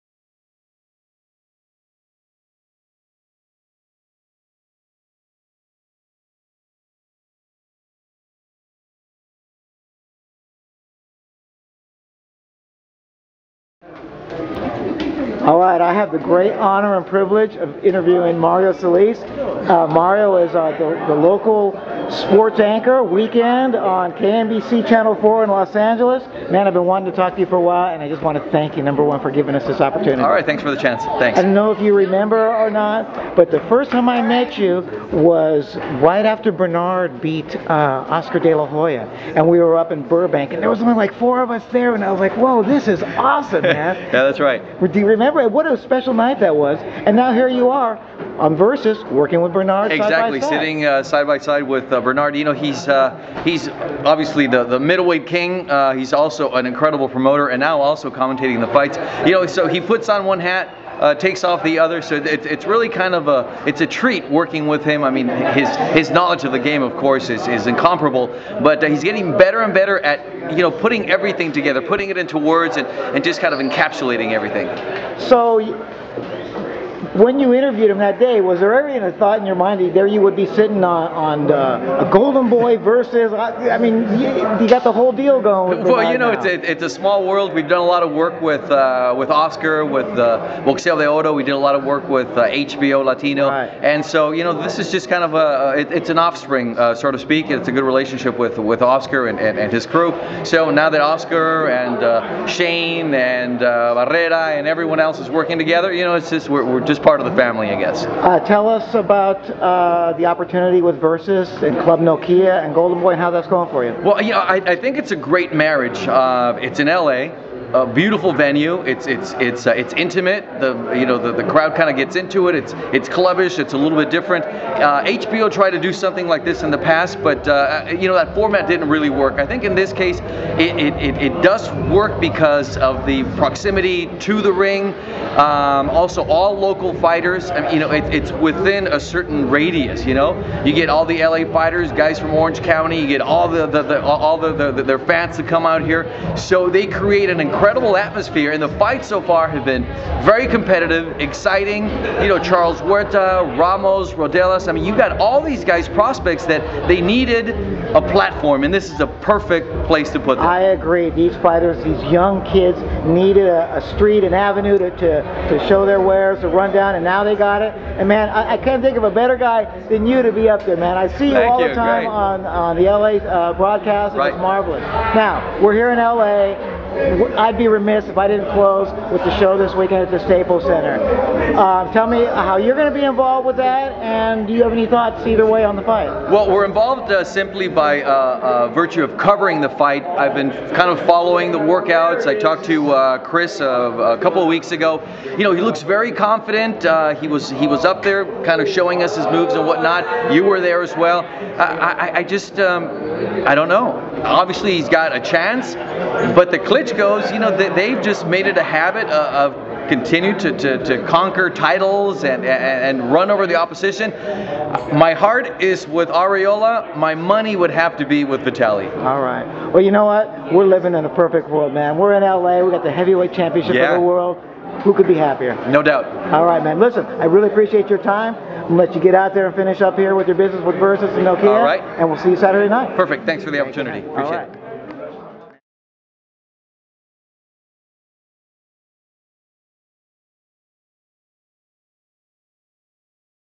Mario Solis One On One Interview
Last night at Club Nokia in Los Angeles Pound4Pound had the great pleasure to speak with the Versus Network "Fight Night" announcer & KNBC 4 Sports Anchor Mario Solis. We are honored to have been able to get Mario's thoughts about his work with Golden Boy as they put on their fine monthly show. Solis has been a long time sportscaster in Los Angeles and since 2002 he has been with KNBC 4 Los Angeles. During that time Solis has seen the likes of Shane Mosley and Oscar De La Hoya represent L.A. and now he will be there when Los Angeles native Chris Arreola takes on Vital Klitschko for the World Heavyweight title.

We spoke to Solis about "Fight Night" on Versus and got his thoughts on this weekends Heavyweight thriller. You don't want to miss what he has to say!